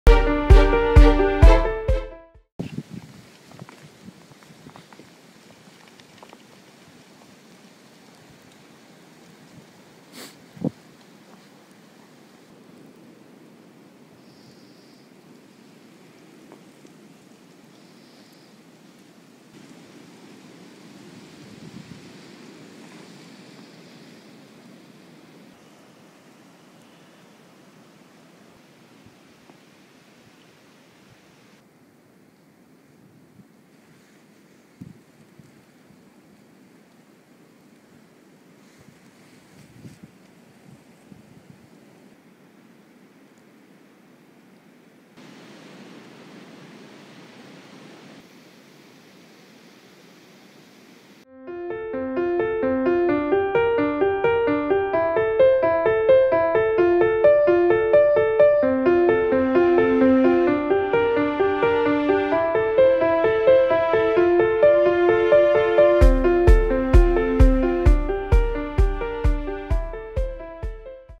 Norfolk Terriers Venturing Out on Day 1 of the Blizzard of 2010
In our blog post we reminisce about the Blizzard of 2010 to teach Ernie about snow.